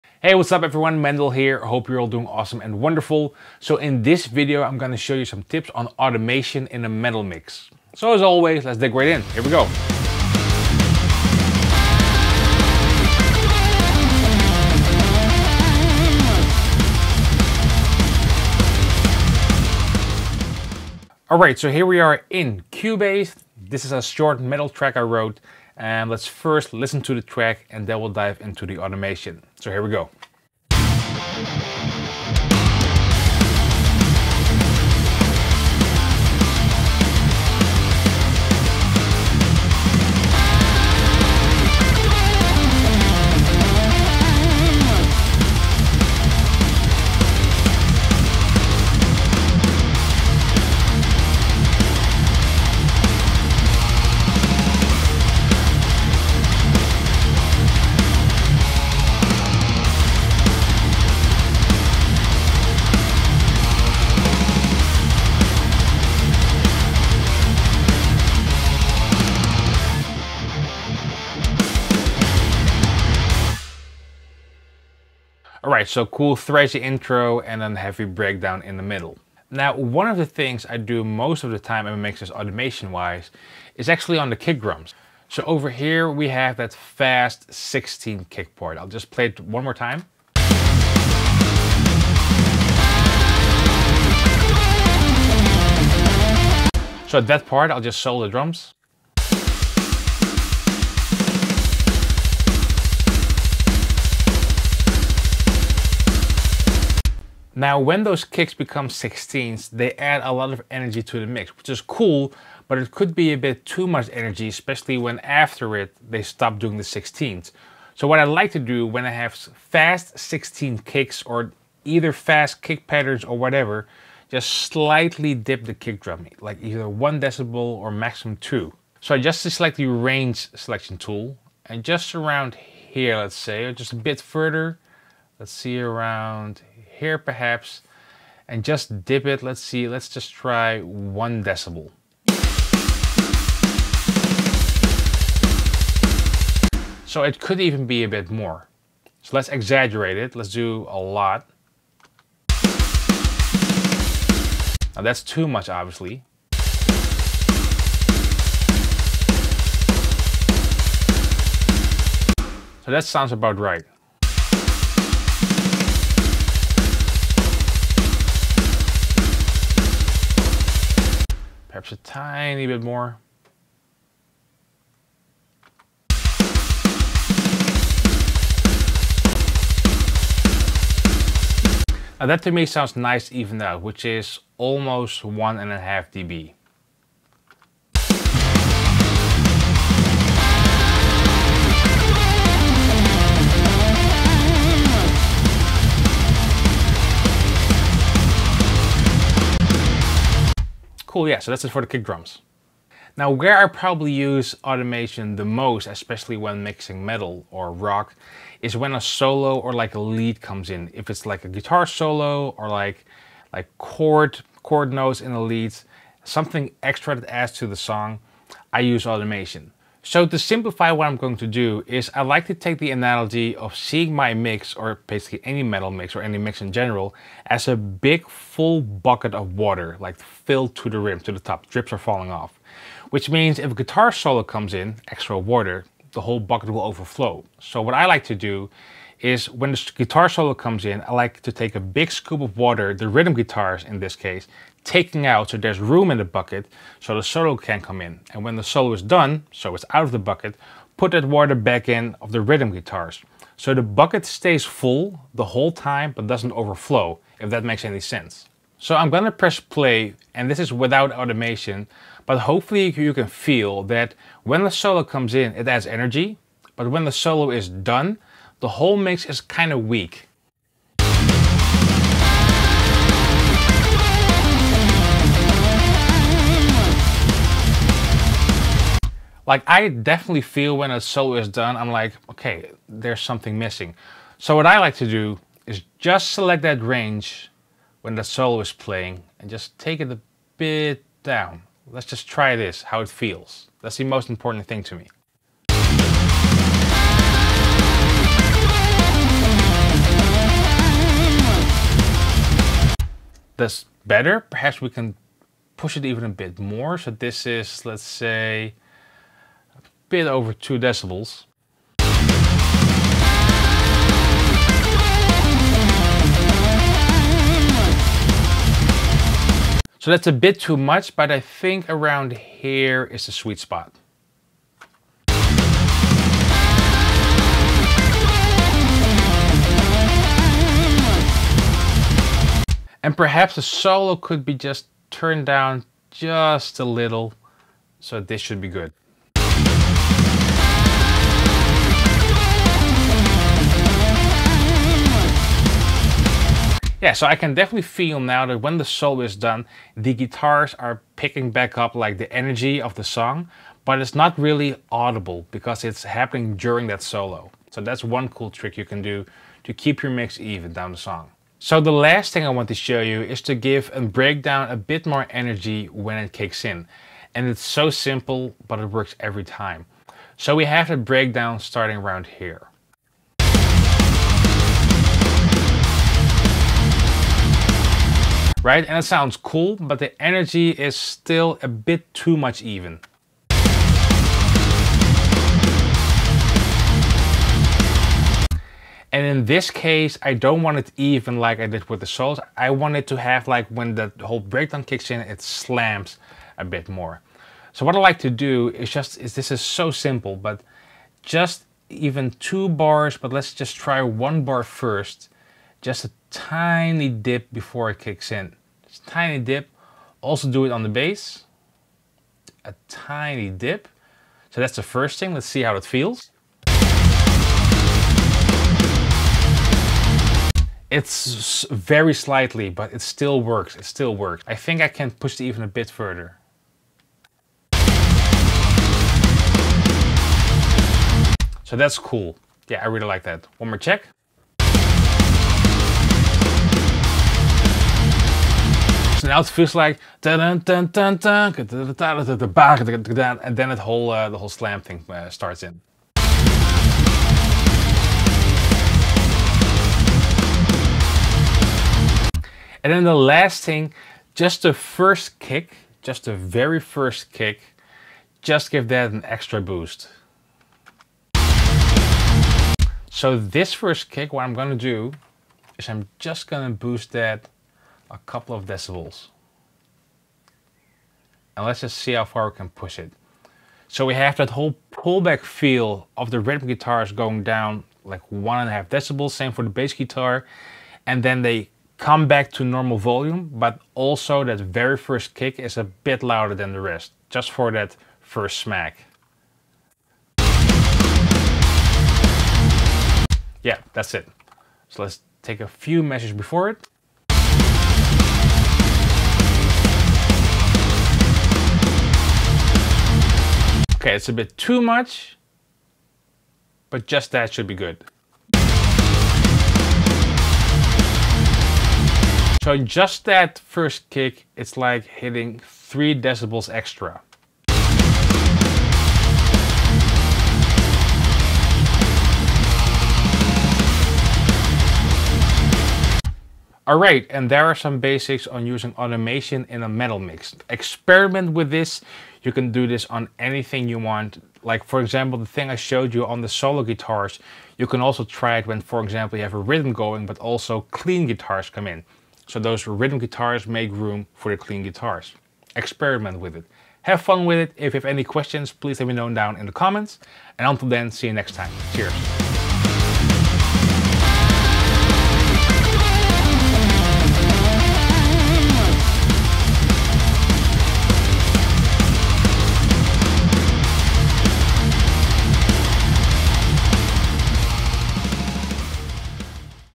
Use Automation to Make Your Metal Mixes Sound Better | Metal Recording Basics in Cubase
Using automation in Steinberg Cubase Pro and Cubase Elements is a good way to improve your metal production. In this Cubase 12 tutorial, Mendel looks at using automation as part of your Cubase workflow for metal mixing. Automation can be used to make your guitar, drums and bass all sit better within your Cubase metal recording, enhancing and reducing the loudness to deliver an optimized mix.